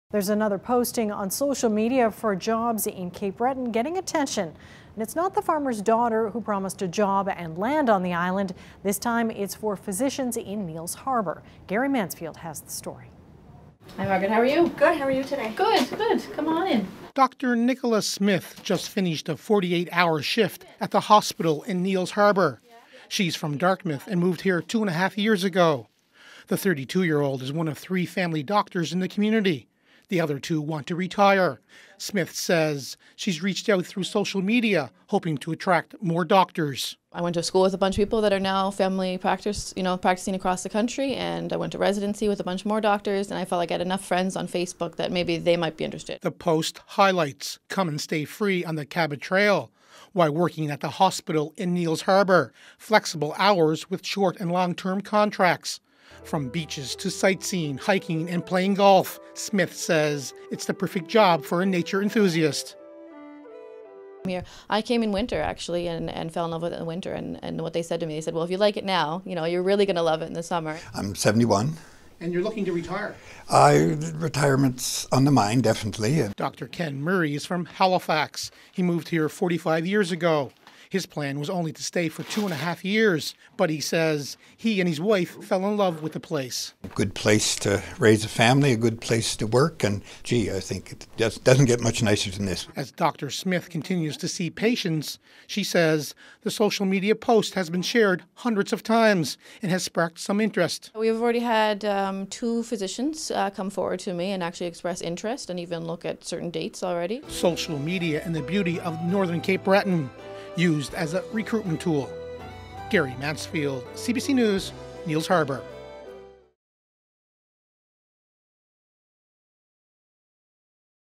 Recruiting on social media: This doctor in Cape Breton turns to Facebook
One of three family physicians in Neils Harbour has turned to social media to boost recruiting efforts and attract other doctors to come to the Cape Breton community.